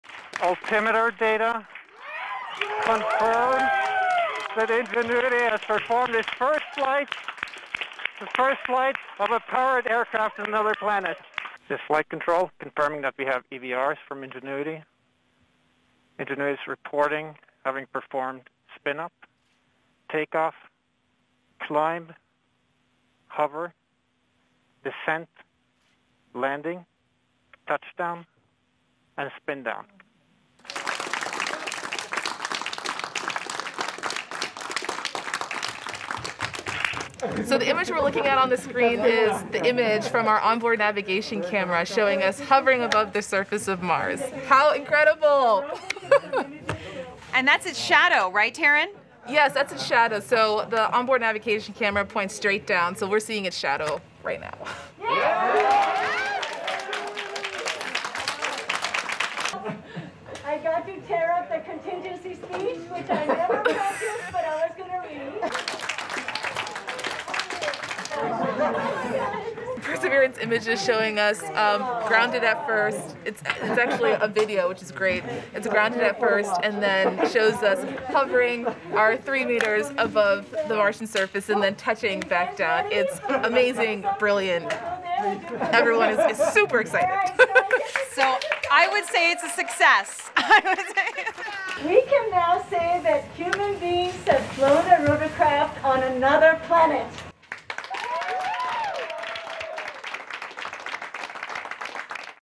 NASA's Mars helicopter Ingenuity makes history
NASA's miniature robot helicopter Ingenuity performed a successful takeoff and landing on Mars early on Monday, achieving the first powered, controlled flight by an aircraft over the surface of another planet, the U.S. space agency said.